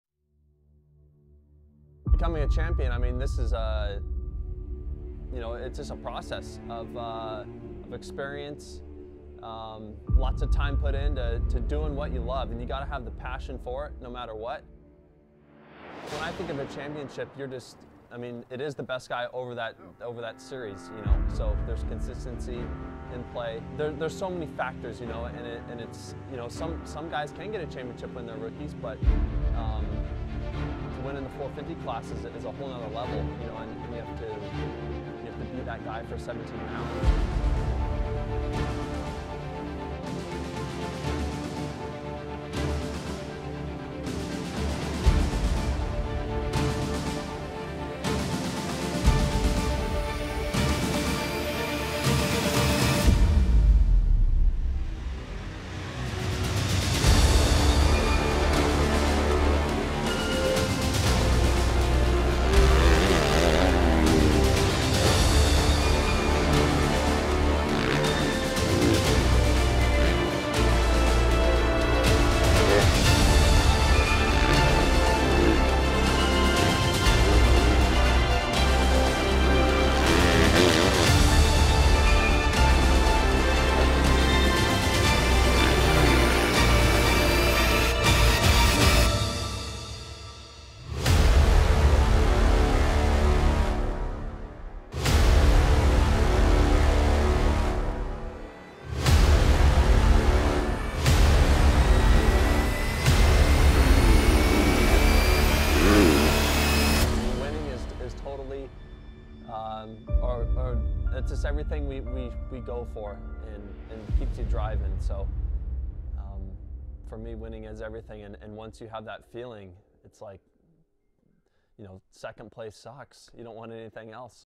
Eli Tomac x Vurb Moto Edit Contest
I had a great time putting this video together and of course what better brands to work with Star Yamaha and Vurb Moto!